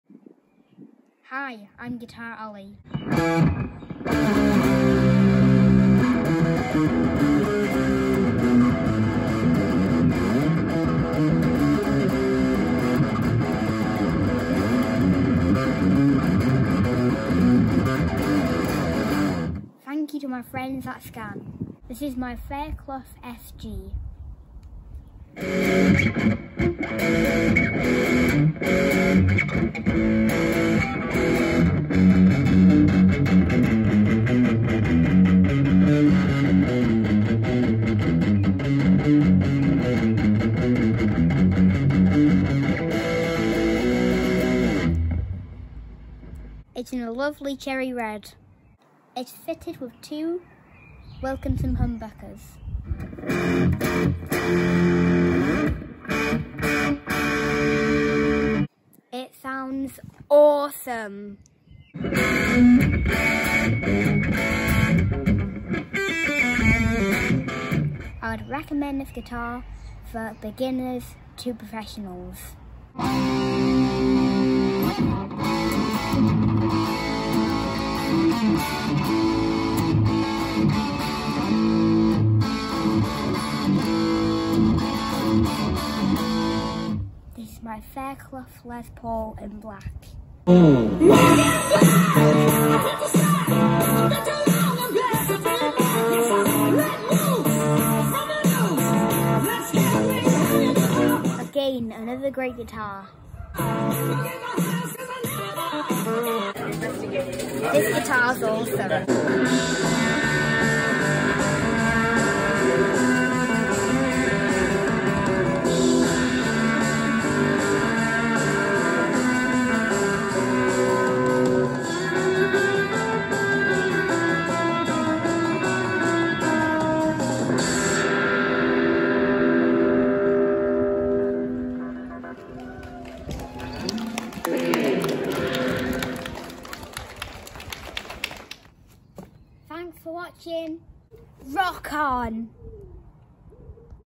Guitar reviews / Les Paul / SG / Fairclough Guitars / Scan Pro Audio / awesome kid guitarist / rock
Olly reviews the awesome Fairclough Guitars.
Black Les Paul style and red SG style.
Super quality for beginner and experienced guitarists.
Both suit all ranges of music. They would make a great addition to any collection.

Thank you Scan Pro Audio / Fairclough Guitars for these fantastic guitars.

AC/DC - Ozzy Osborne - Metallica - Rolling Stones